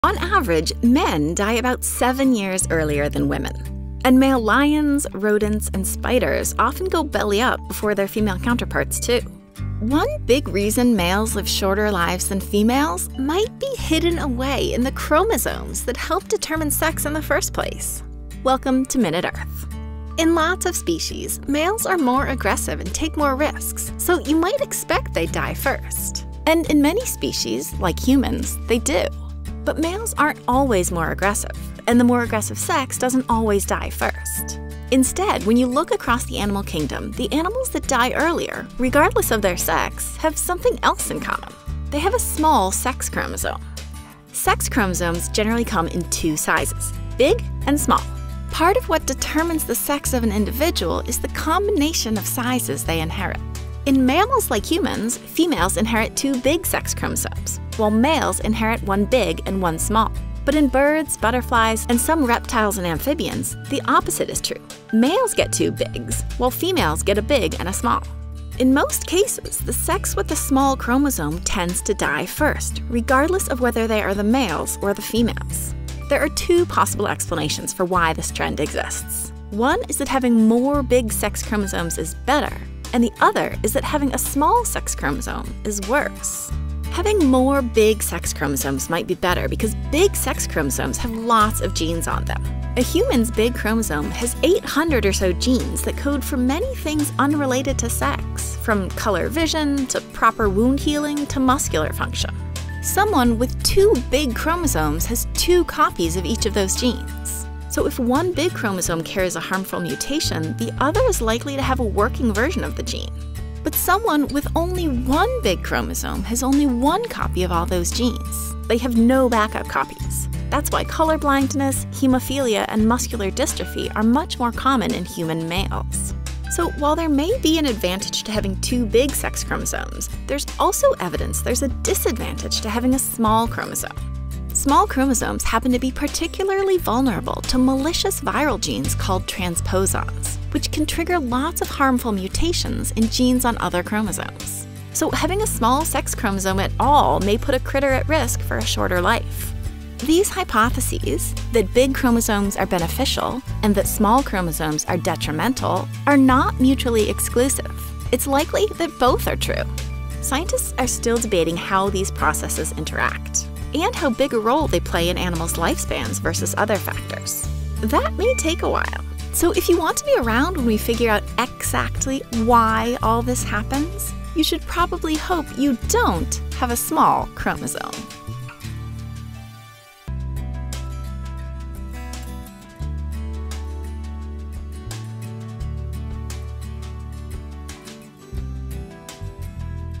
The Actual Reason Men Die First
Because females often outlive males, behavior is often blamed - but there is a decent chance our sex chromosomes might be to blame instead.

To learn more about this topic, start your googling with these keywords:
- Chromosome: a threadlike structure of nucleic acids and protein found in the nucleus of most living cells, carrying genetic information in the form of genes.
- Gene: the basic unit of heredity passed from parent to child.
- Mutation: the changing of the structure of a gene, resulting in a variant form that may be transmitted to subsequent generations, caused by the alteration of single base units in DNA, or the deletion, insertion, or rearrangement of larger sections of genes or chromosomes.
- Recessive allele: a type of allele which will not be manifested in an individual unless both of the individual's copies of that gene have that particular genotype.
- Transposon: a chromosomal segment that can undergo transposition, especially a segment of bacterial DNA that can be translocated as a whole between chromosomal, phage, and plasmid DNA in the absence of a complementary sequence in the host DNA.

CREDITS
Cameron Duke | Script Writer and Director
Kate Yoshida | Narrator and Director
Sarah Berman | Illustration, Video Editing and Animation

OUR STAFF
Alex Reich • Henry Reich • Peter Reich • Ever Salazar
Leonardo Souza • Kate Yoshida

REFERENCES
Austad, Steven N., and Kathleen E. Fischer. “Sex Differences in Lifespan.” Cell Metabolism, vol. 23, no. 6, June 2016, pp. 1022–1033, 10.1016/j.cmet.2016.05.019.

Bronikowski, Anne M., et al. “Sex‐Specific Aging in Animals: Perspective and Future Directions.” Aging Cell, vol. 21, no. 2, 23 Jan. 2022, 10.1111/acel.13542.

Nguyen, Alison H., and Doris Bachtrog. “Toxic Y Chromosome: Increased Repeat Expression and Age-Associated Heterochromatin Loss in Male Drosophila with a Young Y Chromosome.” PLOS Genetics, vol. 17, no. 4, 22 Apr. 2021, p. e1009438, 10.1371/journal.pgen.1009438.

Peona, Valentina, et al. “The Avian W Chromosome Is a Refugium for Endogenous Retroviruses with Likely Effects on Female-Biased Mutational Load and Genetic Incompatibilities.” Philosophical Transactions of the Royal Society B: Biological Sciences, vol. 376, no. 1833, 26 July 2021, p. 20200186, 10.1098/rstb.2020.0186.

Sultanova, Zahida, et al. “Genetic Sex Determination and Sex-Specific Lifespan in Tetrapods – Evidence of a Toxic Y Effect.” BioRxiv, 9 Mar. 2020,

Sultanova, Zahida, et al. “The “Unguarded-X” and the Genetic Architecture of Lifespan: Inbreeding Results in a Potentially Maladaptive Sex-Specific Reduction of Female Lifespan In Drosophila Melanogaster.” Evolution, vol. 72, no. 3, 2 Feb. 2018, pp. 540–552, 10.1111/evo.13426.

Sultanova, Zahida. (2022). Personal communication.

Xirocostas, Zoe A., et al. “The Sex with the Reduced Sex Chromosome Dies Earlier: A Comparison across the Tree of Life.” Biology Letters, vol. 16, no. 3, Mar. 2020, p. 20190867, 10.1098/rsbl.2019.0867.

Xirocostas, Zoe A. (2022). Personal communication.